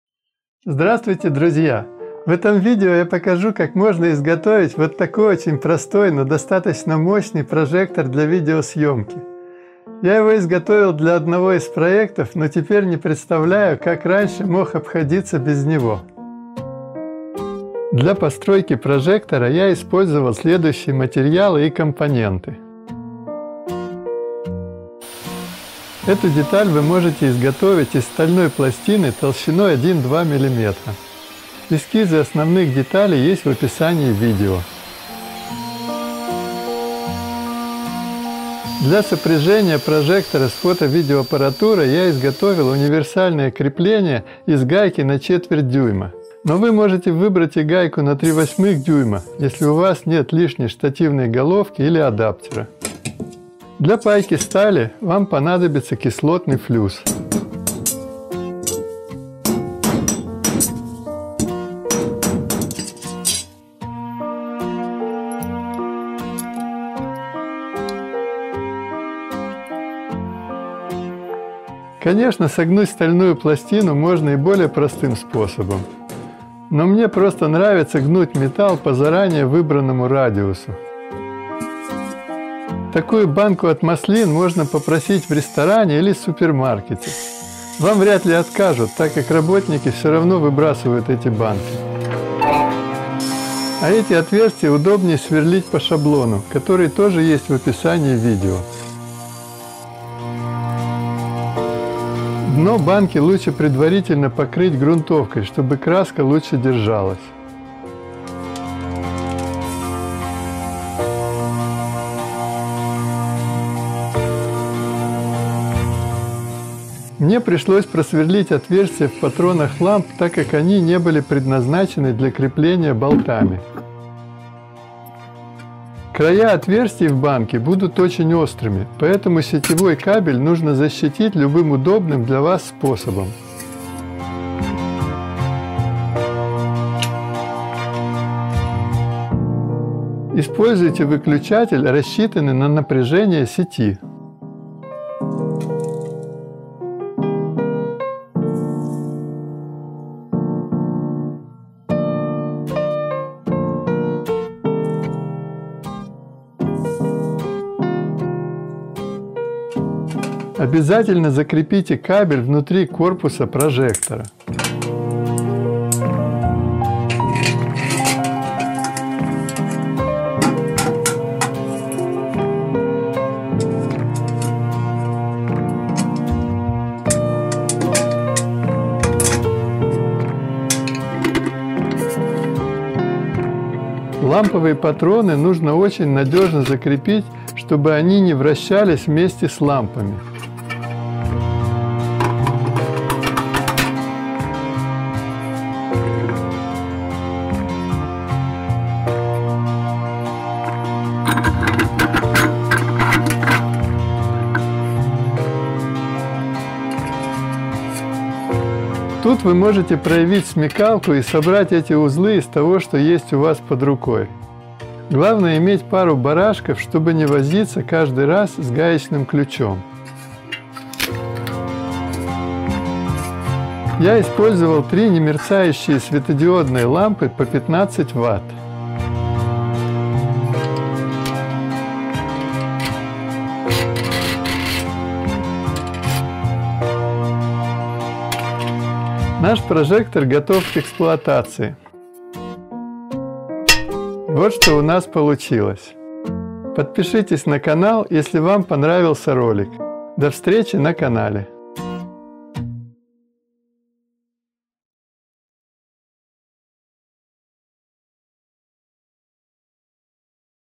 Прожектор для видео своими руками
Я изготовил этот прожектор для лабораторного эксперимента по тестированию защитных масок и фильтров. Но как оказалось, что он очень удобен и для обычной видеосъёмки. Я пока не изготовил шторки для этого прожектора, так как у нас закрыты все магазины промышленных товаров, а у меня нет некоторых компонентов. Так что во время тестирования масок я использовал временные шторки из чётного картона. Надеюсь, когда магазины откроются, я смогу купить всё необходимое и изготовить постоянные шторки для этого прожектора. Но и сейчас прожектом уже можно успешно пользоваться. Металлическая банка очень хорошо рассеивает тепло, поэтому лампы внутри прожектора не перегреваются.